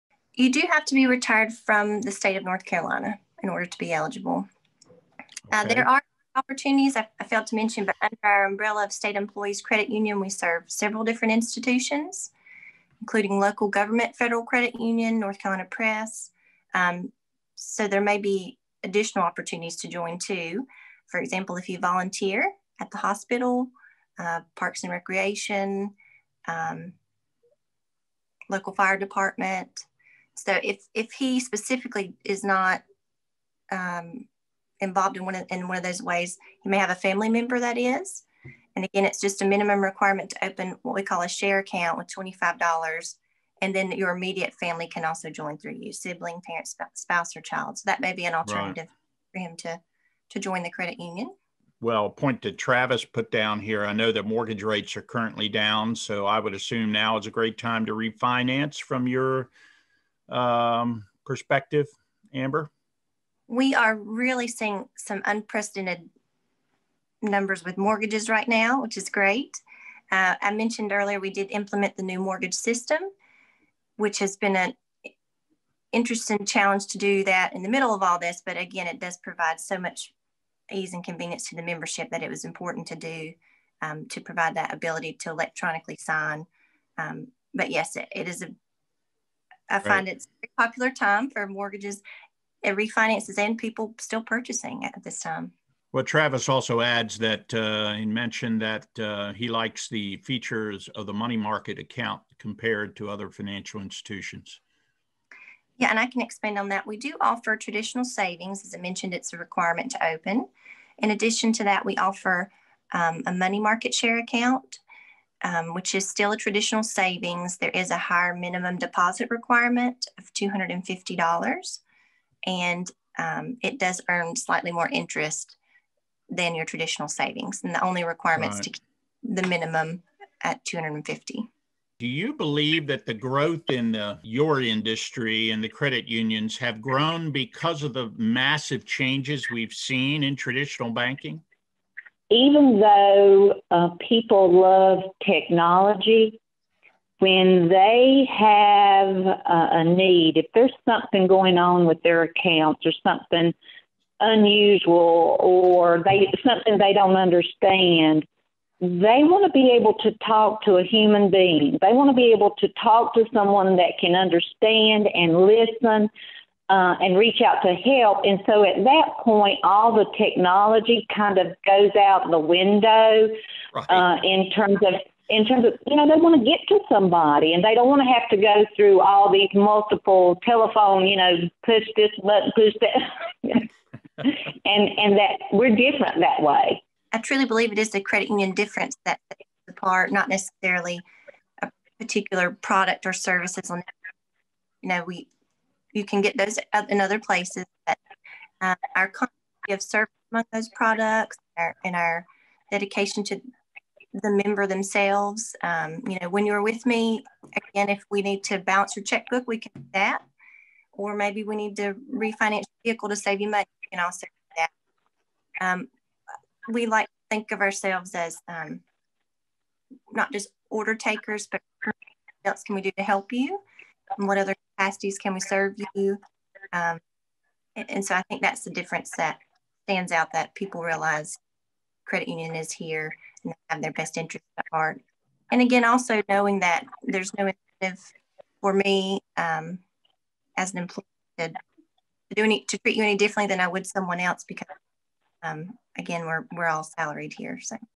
Virtual Mixer #32 - State Employees' Credit Union, Amber Fleming (Part 2 of 2)
The Greater Mount Airy Chamber of Commerce held a Virtual Mixer on November 5, 2020. Guest Speaker, Amber Fleming, Senior Financial Services Advisor at State Employees' Credit Union shared details on how a credit union differs from a regular bank, services that they can provide, eligibility requirements, and a general update on how the organization is currently operating during the COVID-19 pandemic. We also briefly heard from SECU Senior Vice President, Lilnette Phillips.